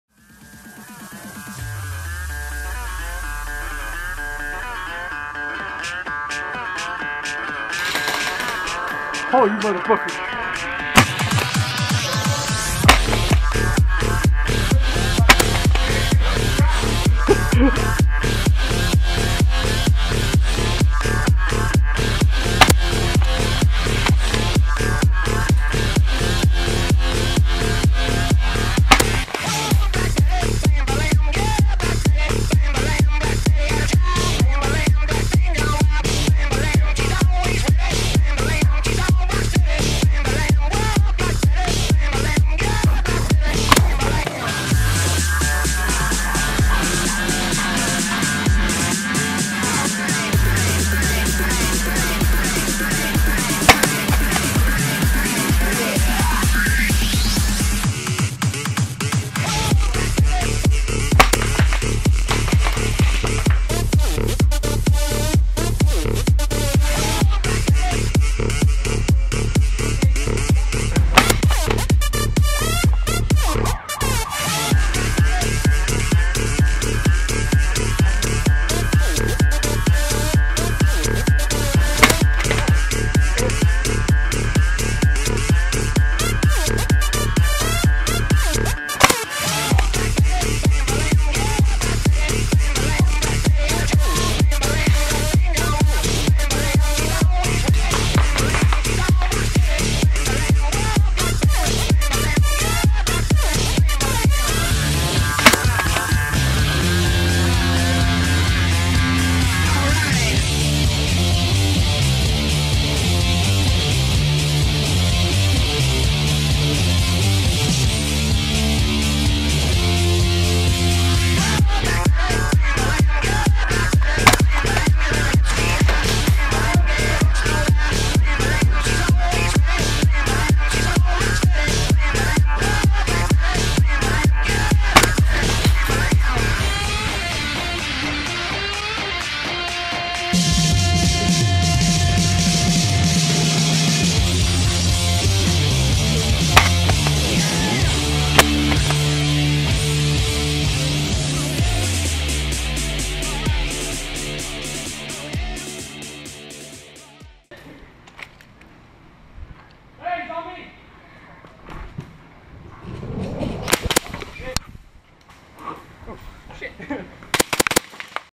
CJ's Shotgun CQB
Some footage of CJ and his shotgun at Airsoft Columbia.